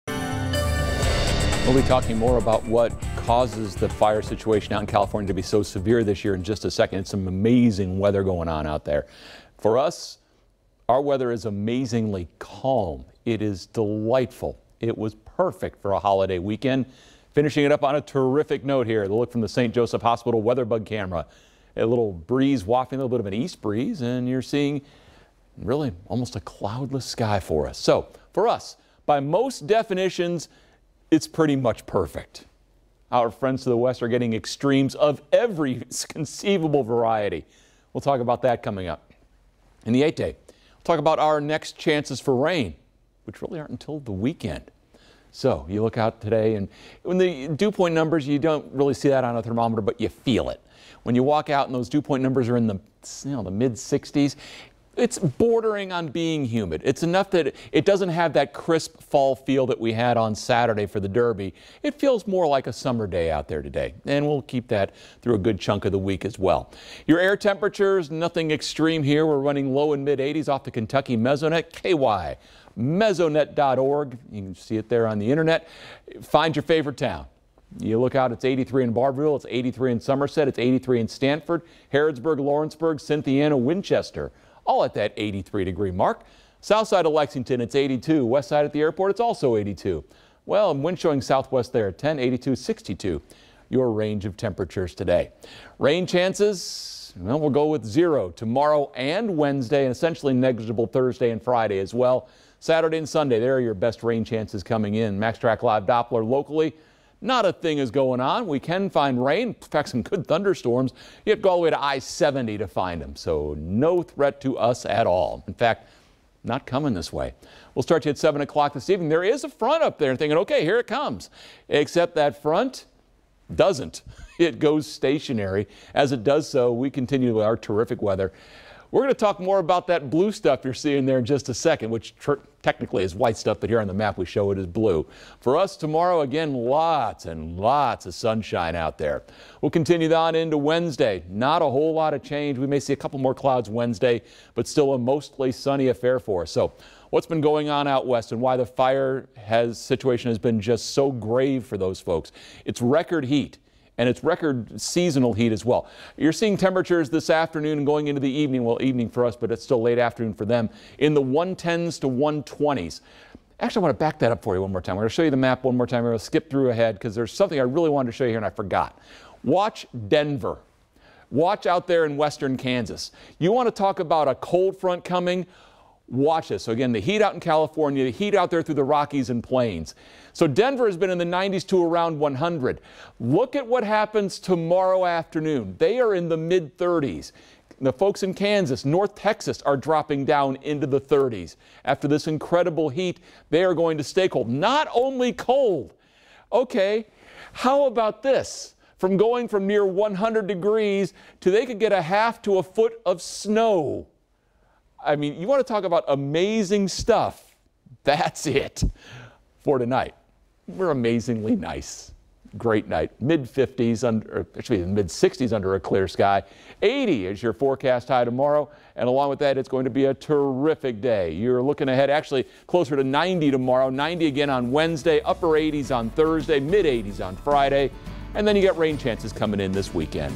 Bill Meck's 6pm weather forecast
After a glorious Labor Day weekend, there is little sign of any significant change to our overall weather pattern.